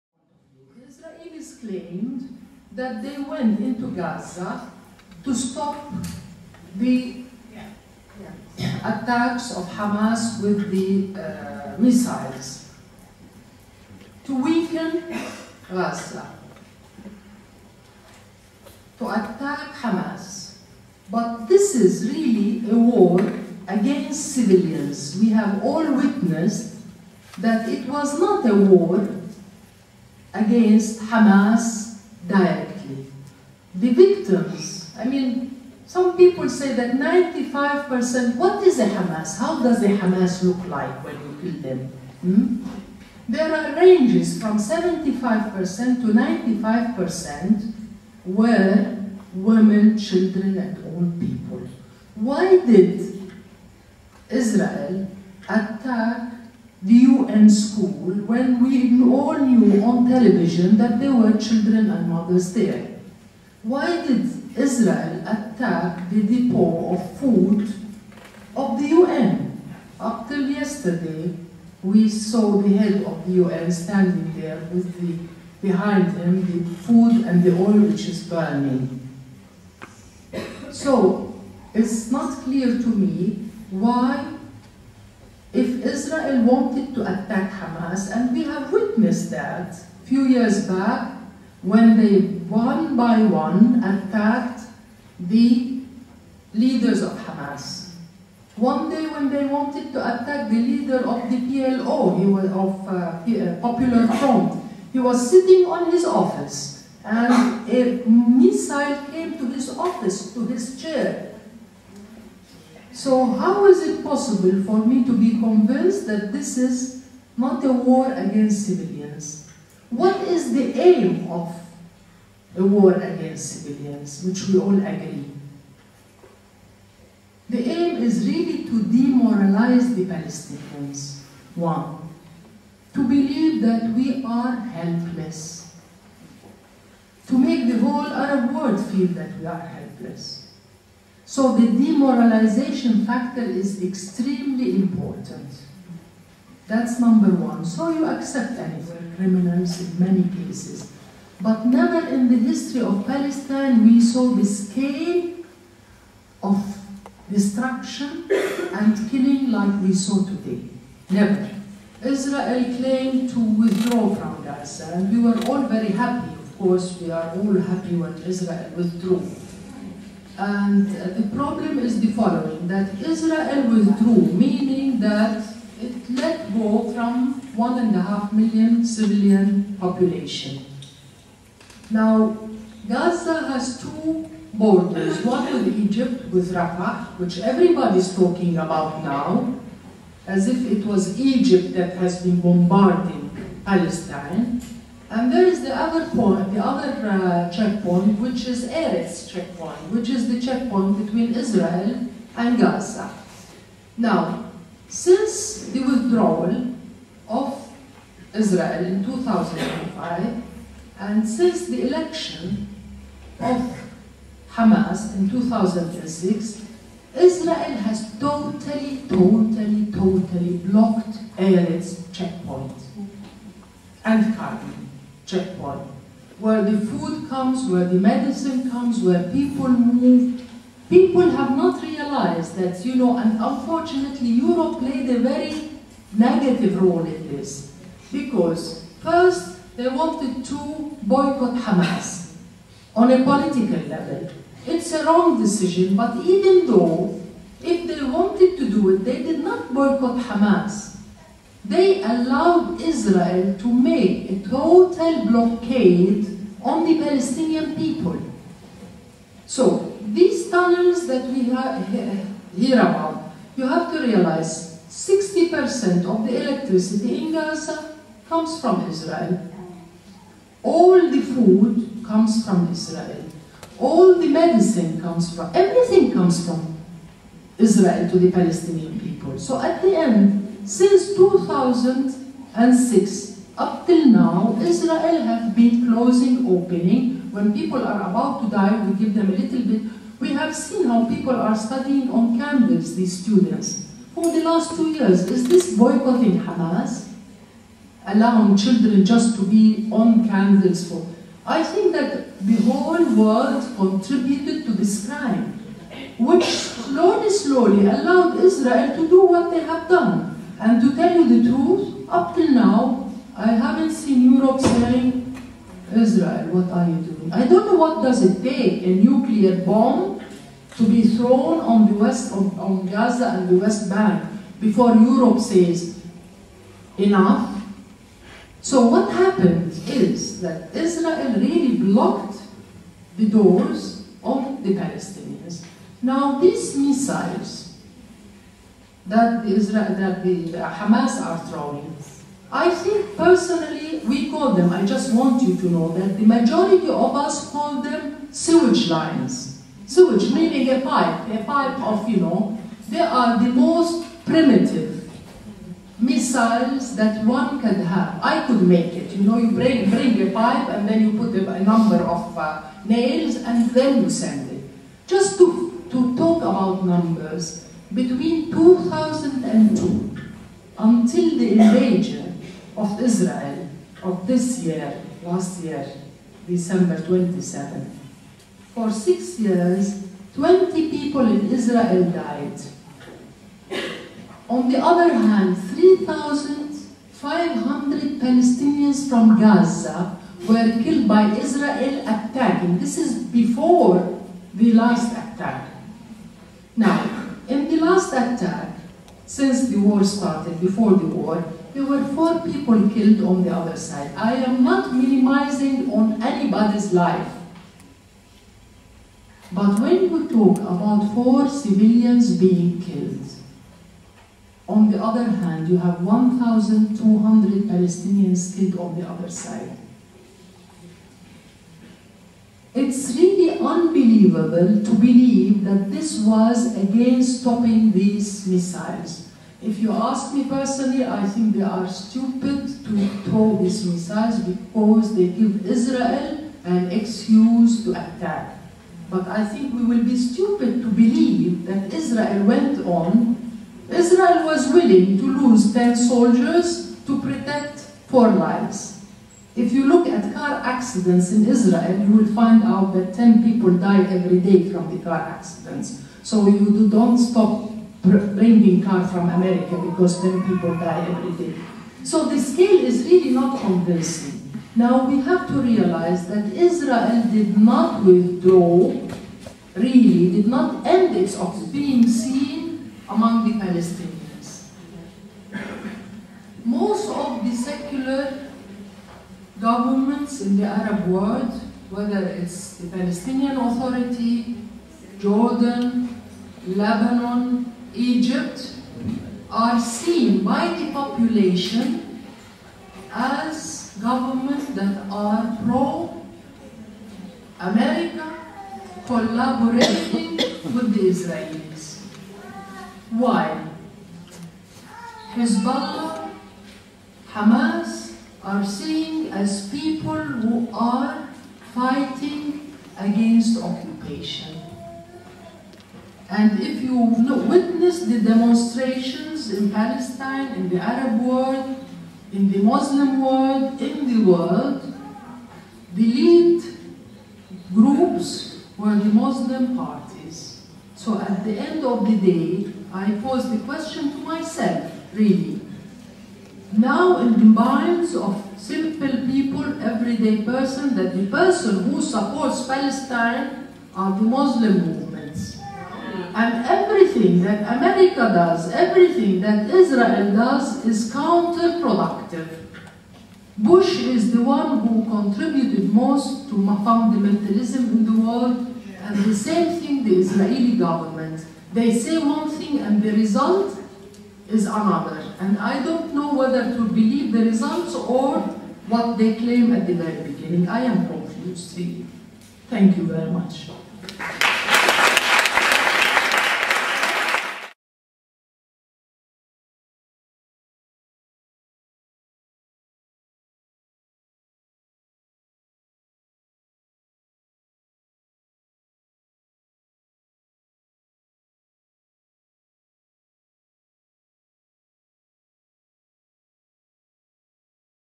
Suad Amiri (parte II)
Suad Amiri, Casa Internazionale delle Donne, Roma. Gennaio 2009, presentazione romanzo: "Niente sesso in città".
سعاد عامري غزة حماس فلسطين أداب لغة العربية